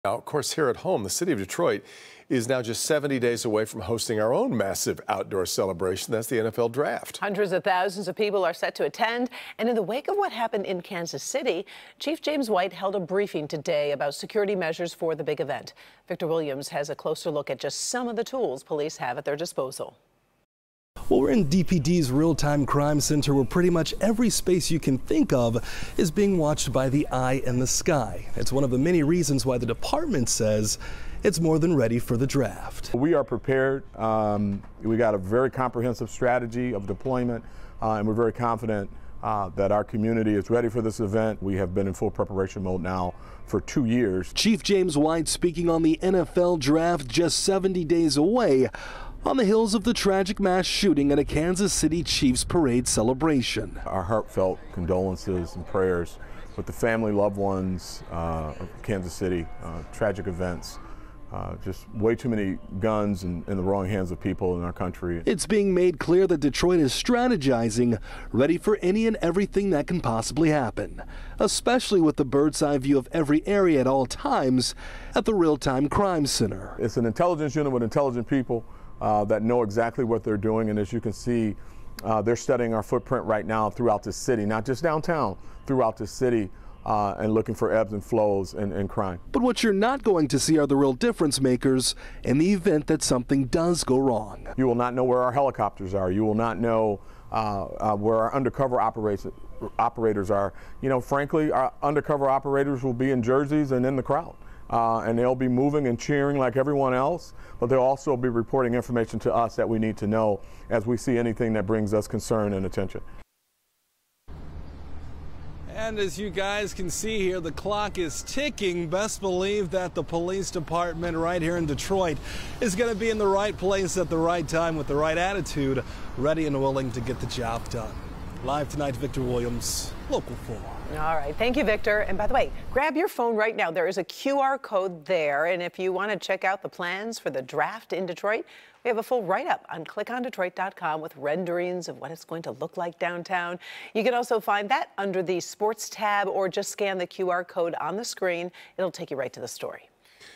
Detroit Police chief assures safety measures in place for 2024 NFL Draft
The City of Detroit is just 70 days away from hosting a massive outdoor celebration: the 2024 NFL Draft.